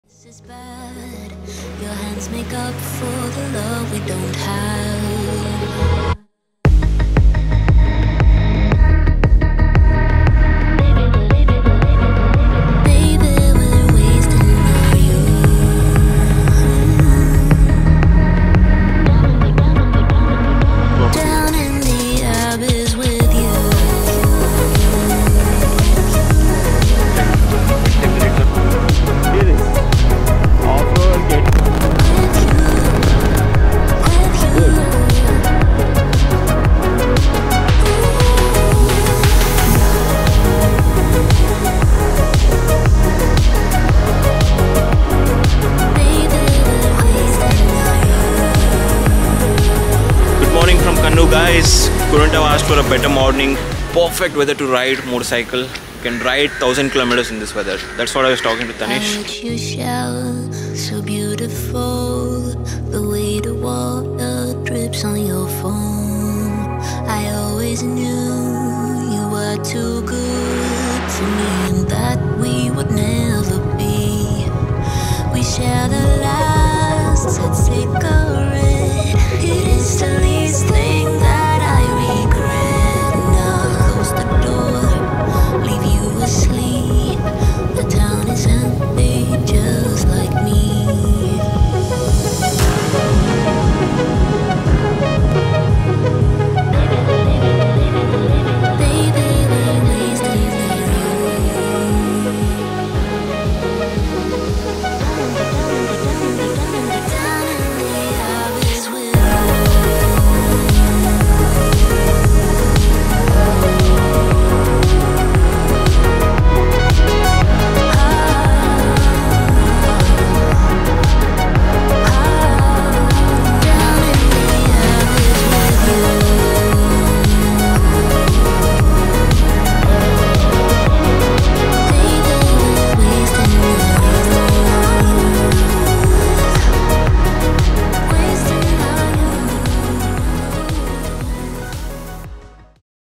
The Ride to WESTERN GHATS ! (trailer)
Join us on an unforgettable journey as we trade bug fixes for breathtaking vistas in the Western Ghats of India! After endless weekends chained to our desks, we're breaking free on two wheels, rediscovering the joy of the open road. From unexpected detours to finding serenity amidst the chaos, this motorcycle escapade is a tale of two software engineers embracing life beyond the code. Buckle up and come along for the ride on Wiseman Travel – where the adventure begins at the end of the road